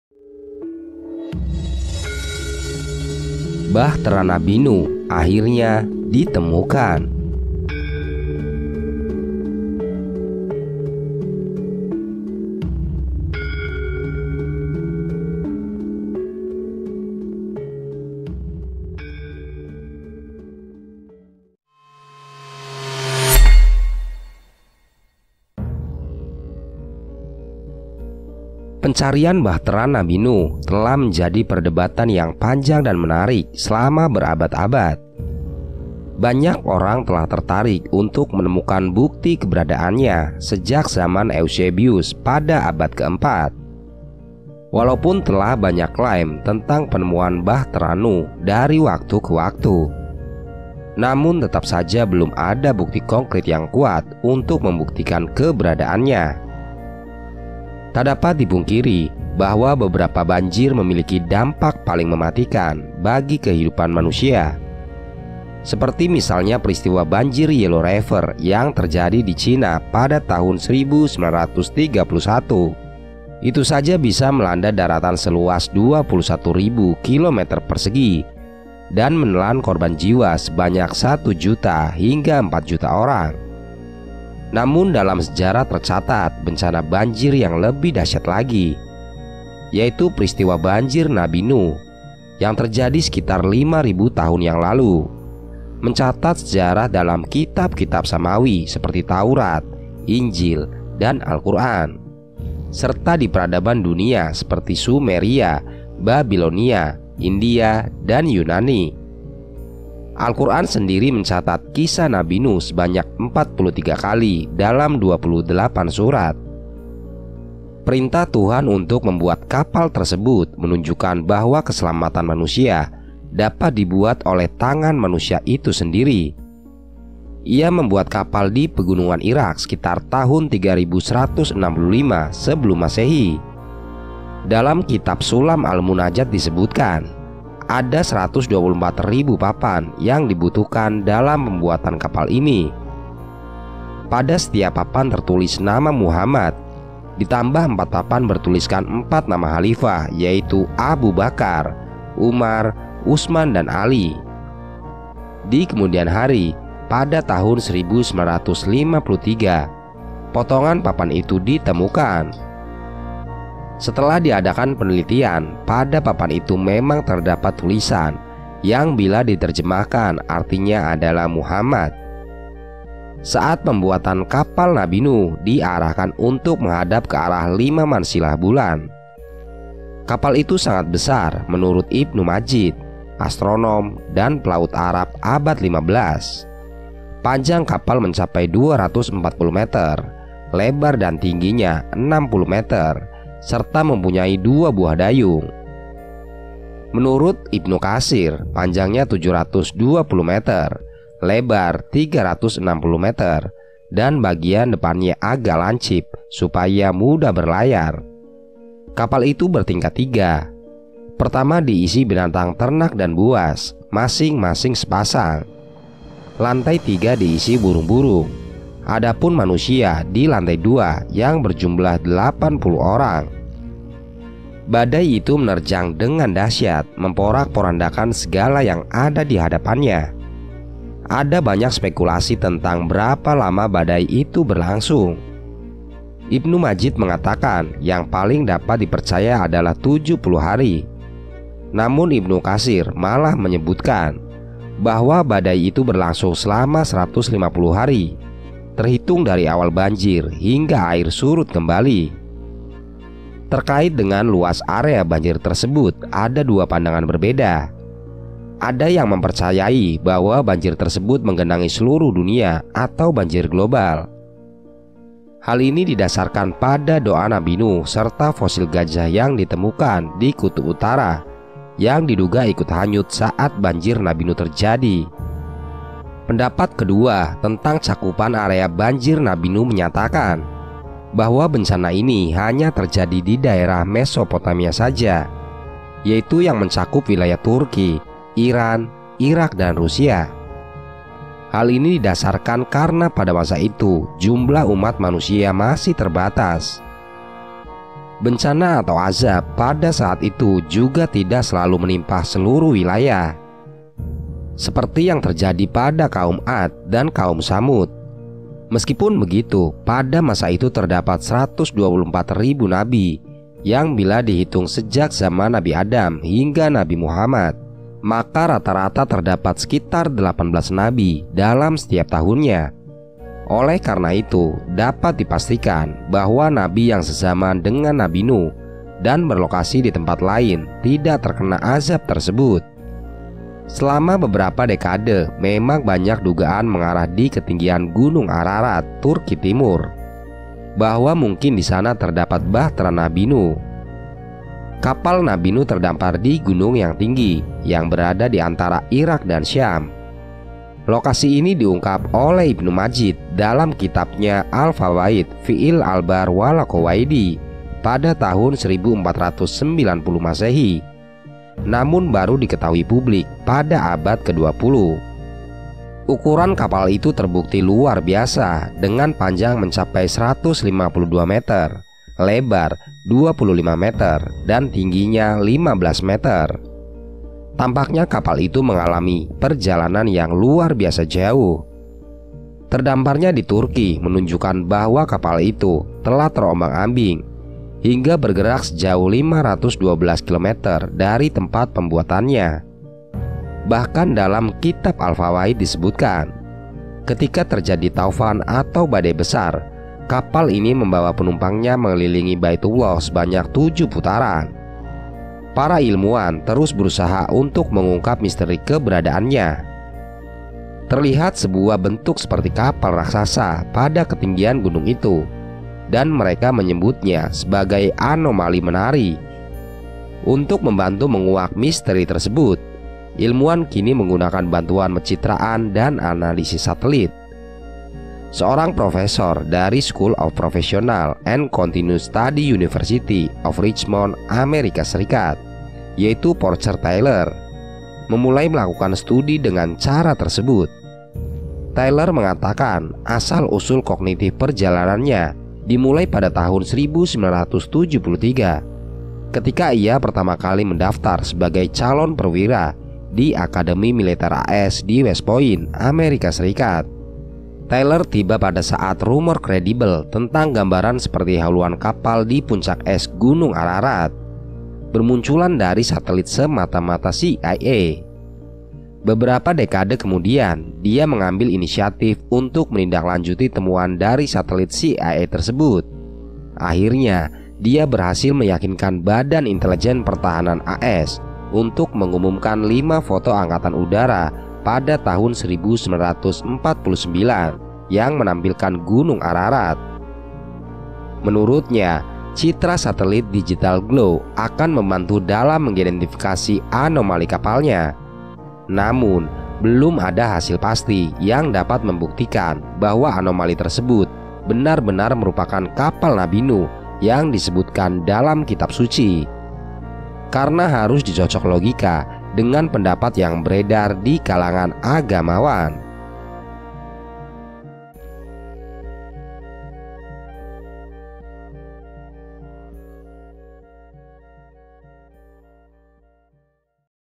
DUNIA TERCENGANG ❗️❗️ KAPAL NABI NUH DITEMUKAN - ILMUWAN SYOK MELIHAT TULISAN PADA PAPANNYA
pencarian Bahtera Nabi Nuh telah menjadi perdebatan yang panjang dan menarik selama berabad-abad.
banyak orang telah tertarik untuk menemukan bukti keberadaannya sejak abad ke-4.
walaupun telah banyak klaim tentang penemuan Bahtera Nuh dari waktu ke waktu namun tetap saja belum ada bukti konkrit yang kuat untuk membuktikan keberadaannya.

Temuan-temuan Kapal Nabi Nuh. Sudah sejak lama, pencarian bahtera Nuh menjadi isu yang memicu perdebatan panjang di kalangan para peneliti dan sejarawan. Sejak zaman Eusebius pada abad ke-4, upaya untuk menemukan bukti keberadaan bahtera tersebut terus bergulir hingga pada akhirnya muncul berbagai penemuan menarik, termasuk temuan di gunung Ararat di Turki dan replika yang dibuat berdasarkan tablet tanah liat sumeria. Namun, di balik semua itu, ada pertanyaan yang mendasar, apakah bahtera Nuh benar-benar pernah ada? Dalam narasi kali ini, mari kita telusuri fakta dan temuan menarik seputar pencarian bahtera Nuh, dan mari kita bersama-sama mencari jawabannya.
Music